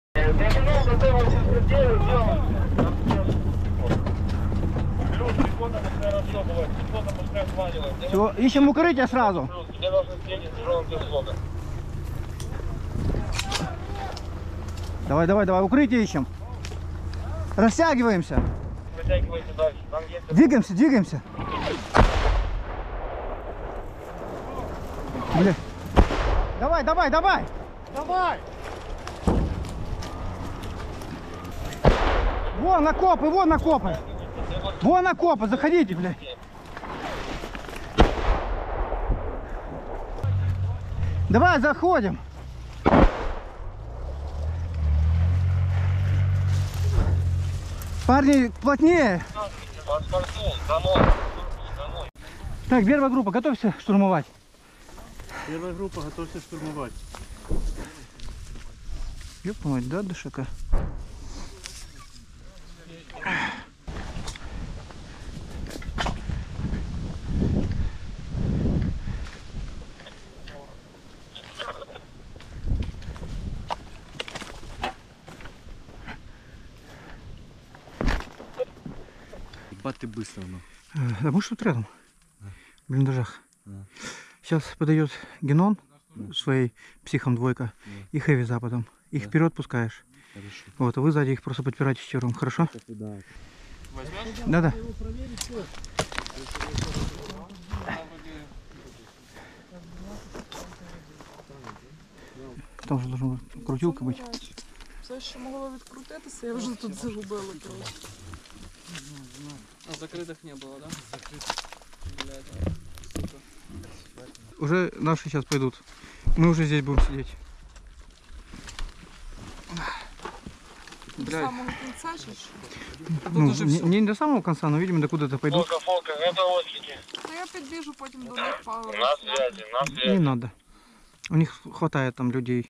💥 Witness the Valor of 3rd Separate Assault Brigade's 3rd Company! 🎥
Despite enemy artillery targeting the forest strip, these fearless fighters stormed the "green house," driving out the occupants and seizing valuable trophies, including a DShK machine gun.